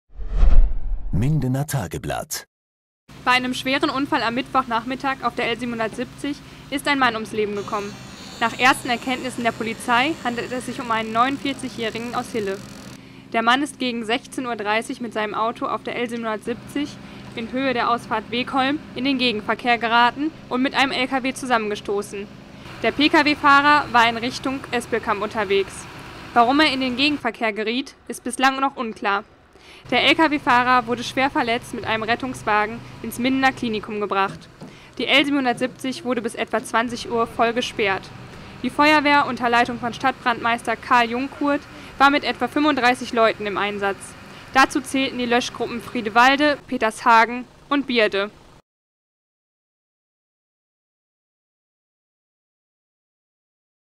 Tödlicher Unfall auf der L 770 in Petershagen
Petershagen (kra). Auf der L 770 in Höhe der Ausfahrt Wegholm hat es am Mittwochnachmittag einen schweren Unfall gegeben, bei dem ein Mann um's Leben kam.